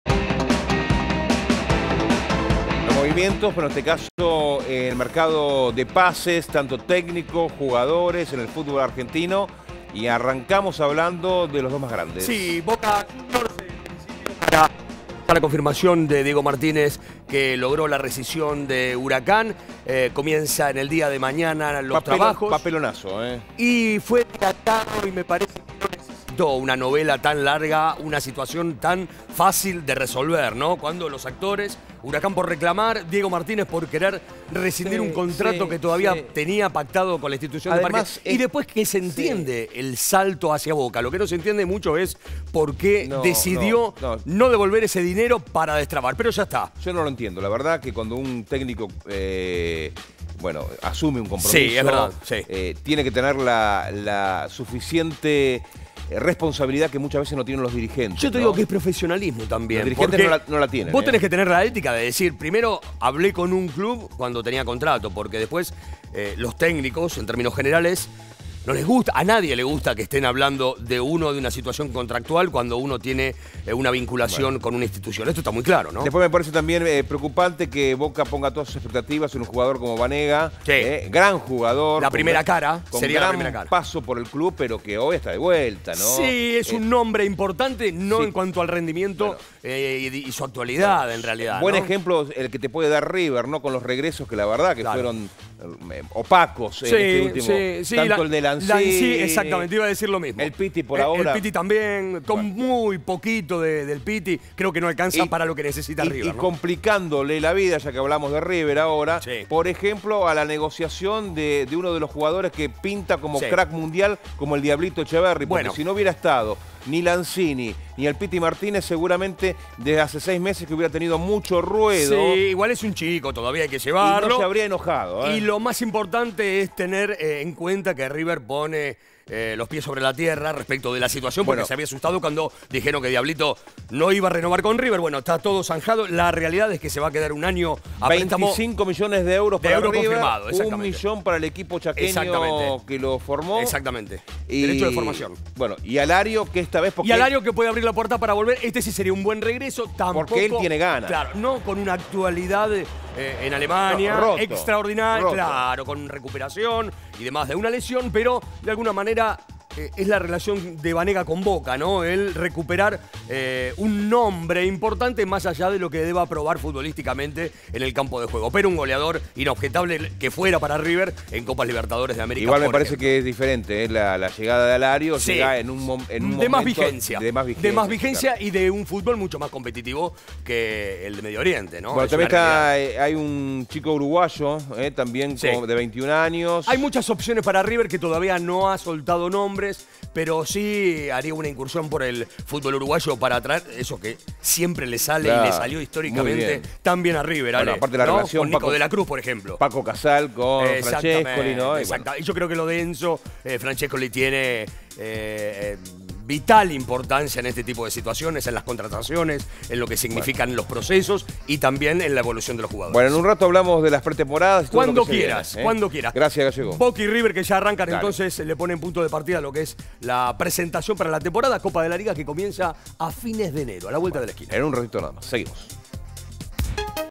El mercado de pases de Boca y River
Boca y River se renuevan de cara al inicio de la temporada. Incluso el Xeneize lo hará con nuevo entrenador: Diego Martínez.